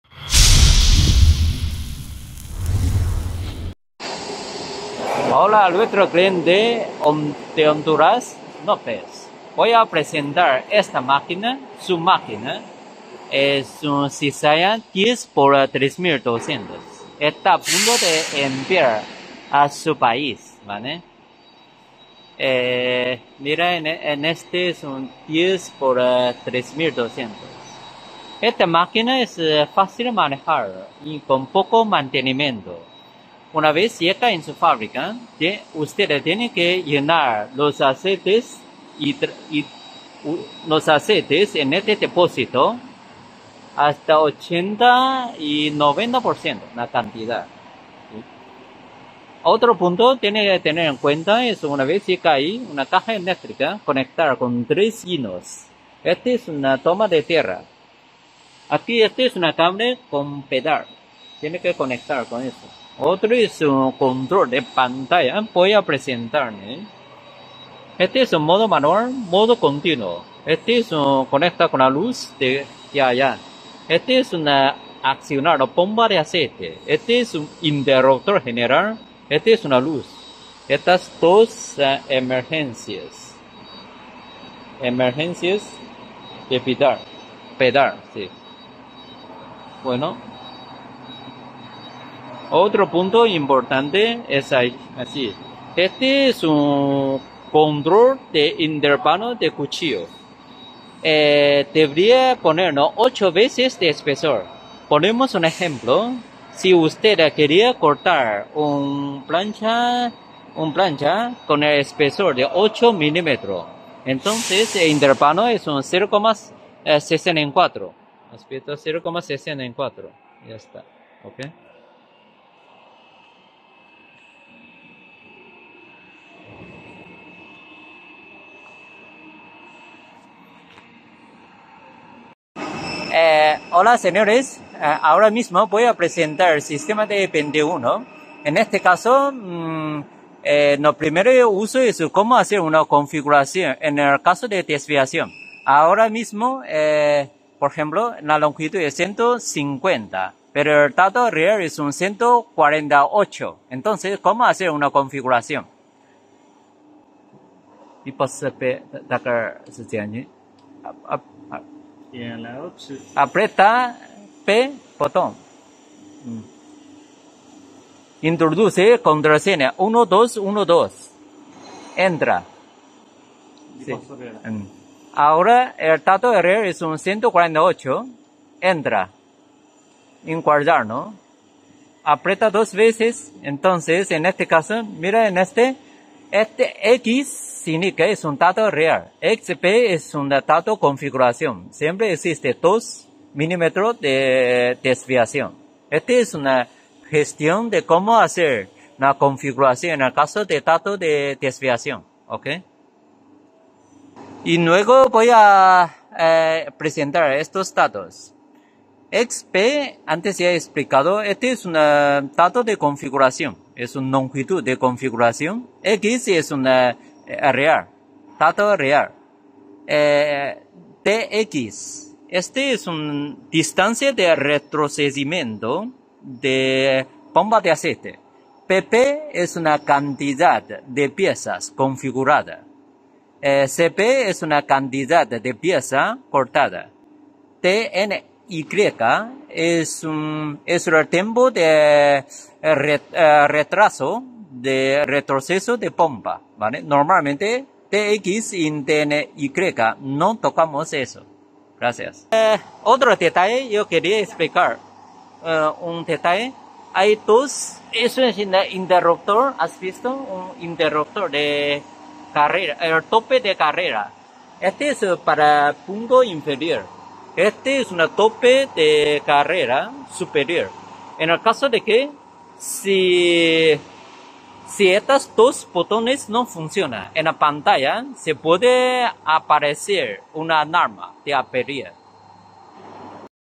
Video de swing cizalla 10mm3200 para el cliente hondureño.Hidráulica Guillotina.Cortadora Hidráulica
Video de swing cizalla 10mm3200 para el cliente hondureño.Hidráulica Guillotina.Cortadora Hidráulica.Cizalla hidráulica.Hidráulica Cizalla.máquina de guillotina hidráulica
Fabricante: MÁQUINA CNC JUGAO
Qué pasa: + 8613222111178